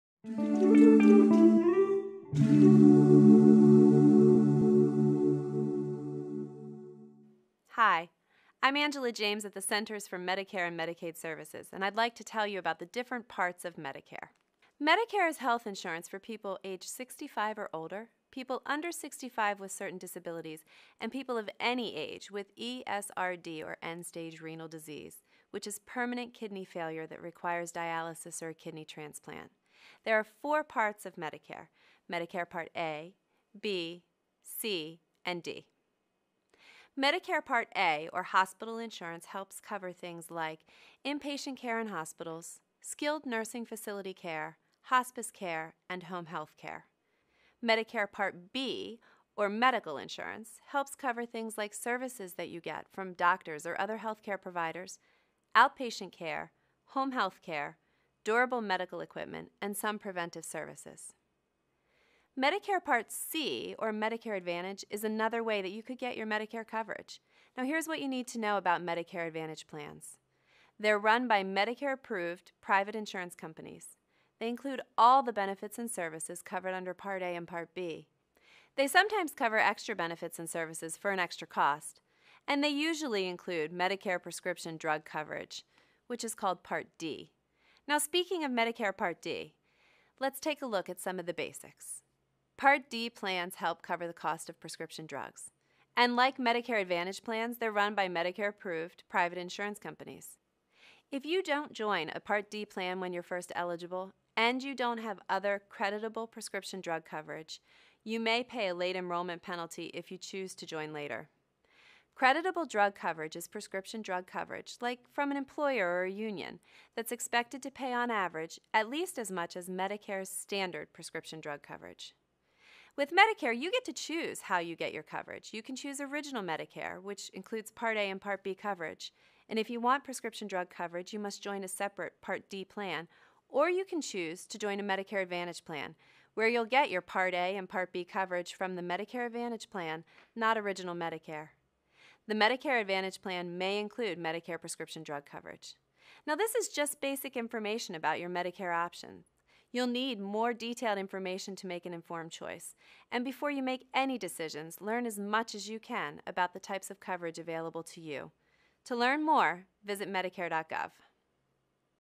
Medicare & You Different Parts Of Medicare
Angela James from the Centers for Medicare and Medicaid Services explains the different parts of Medicare and who is eligible.